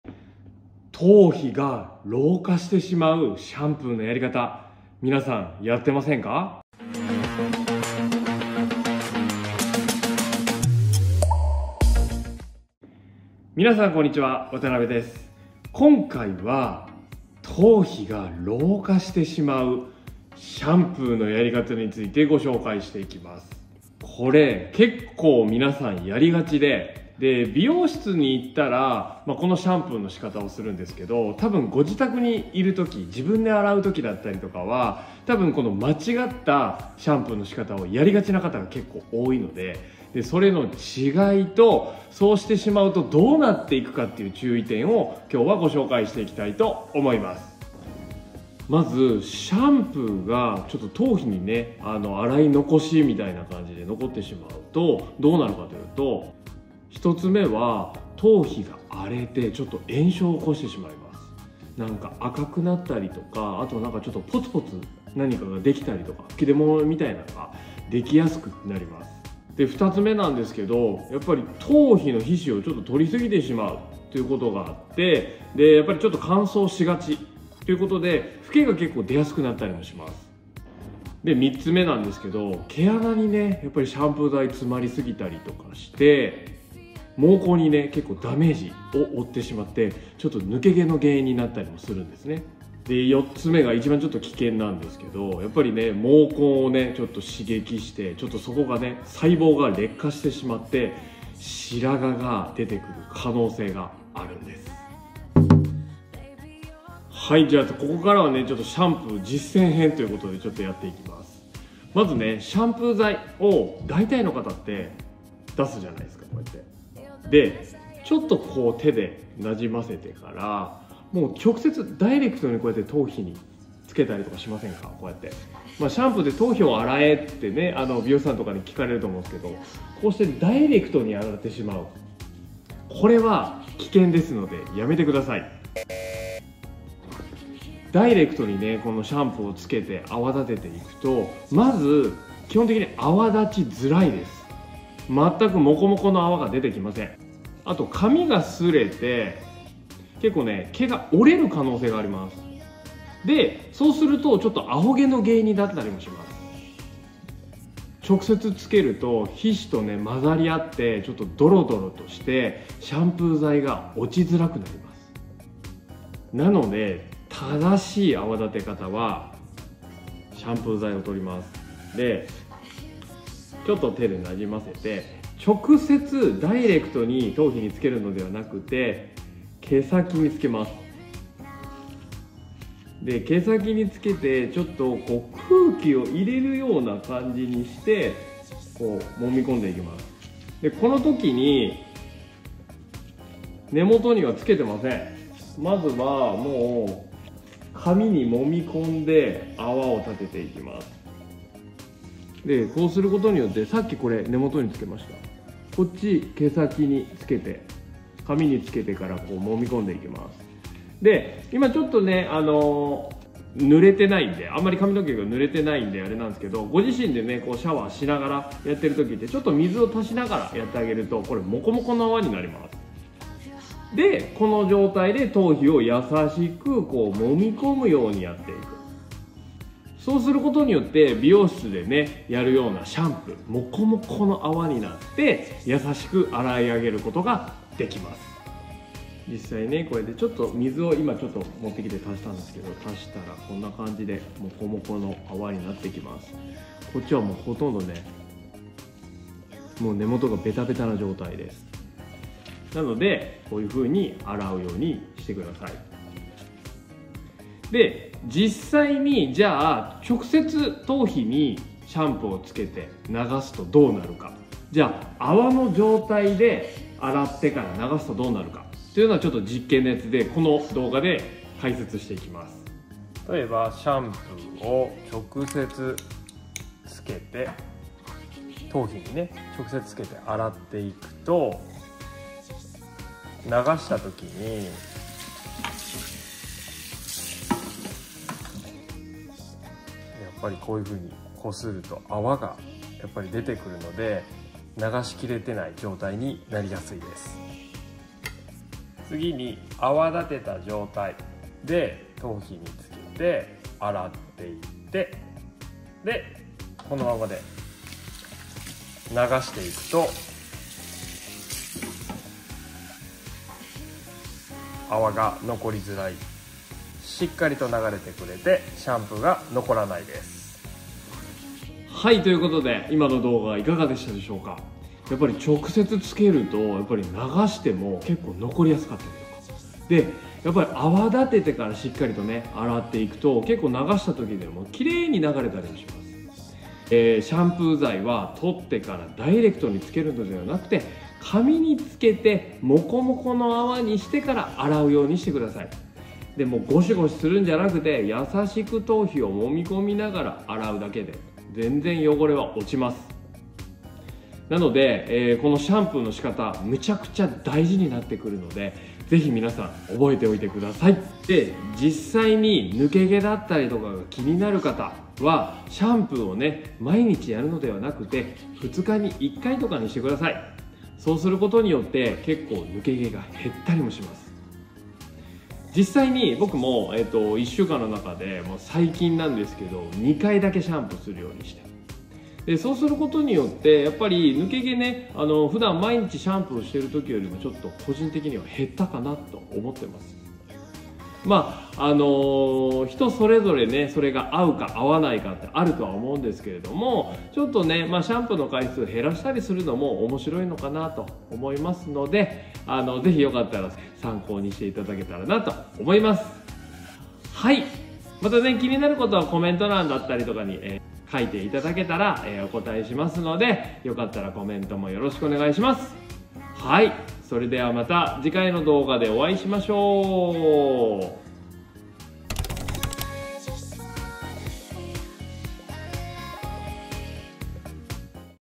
⚠️絶対にやめて⚠️シャンプー剤を直接頭皮につけると白髪、抜け毛が増えます。。
今回は、髪を洗うときに一番気を付けてほしい洗い方についてご紹介します。

間違った洗い方をすると、意外とシャンプーは頭皮に残りやすかったりして

白髪、抜け毛の原因になります。